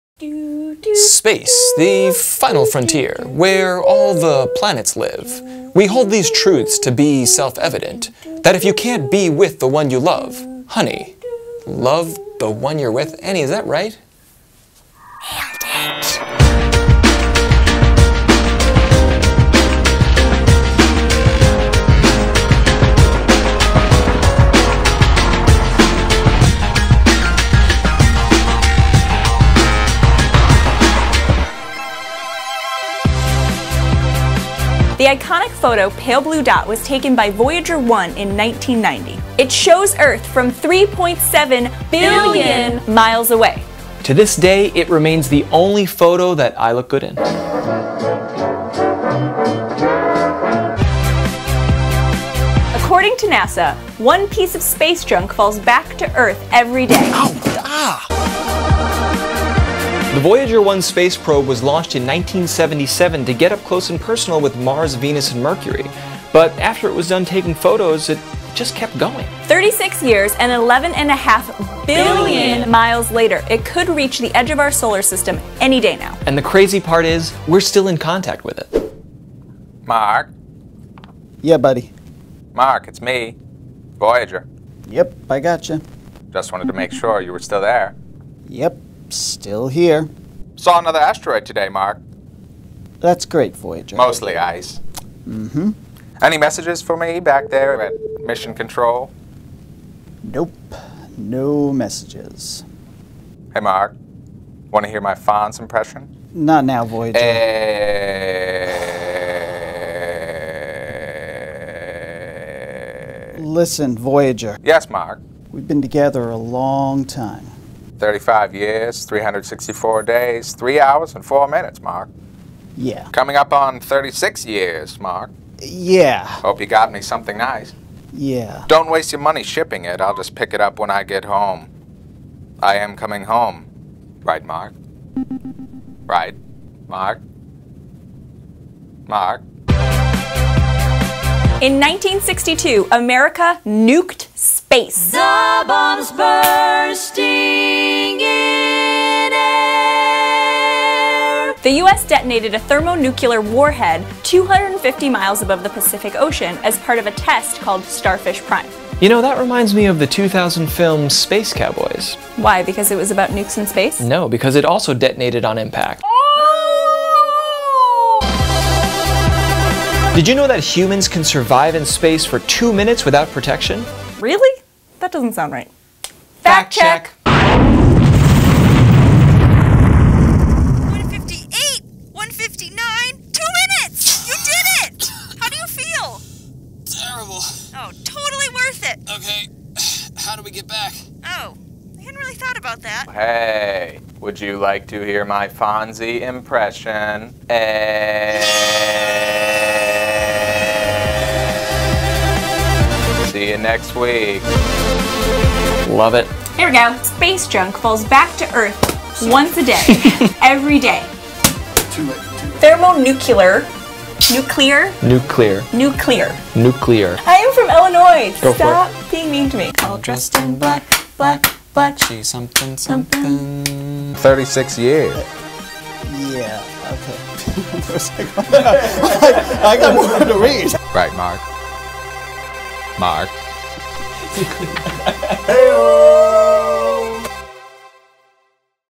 5 Amazing Facts About Outer Space
Space is big. REALLY big. Some scientists estimate it may even be bigger than One Direction.

Follow Matt & Annie, and send them your ideas for 5facts!

*THE FACTS*
1. The Pale Blue Dot

2. Space Junk

3. Voyager 1

4. Starfish Prime

5. Two Minutes

*CREDITS*
Hosted by Matt Silverman and Annie Colbert.
Mission Control Mark played by Lance Ulanoff.
Voyager 1 played by Max Knoblauch.
Written by Annie Colbert, Evan Engel, Matt Silverman and Max Knoblauch.
Post-production by Evan Engel.
Camera Assistance and graphics by Bianca Consunji.
5facts logo and additional graphics by Emil Lendof.
Edited and Directed by Matt Silverman.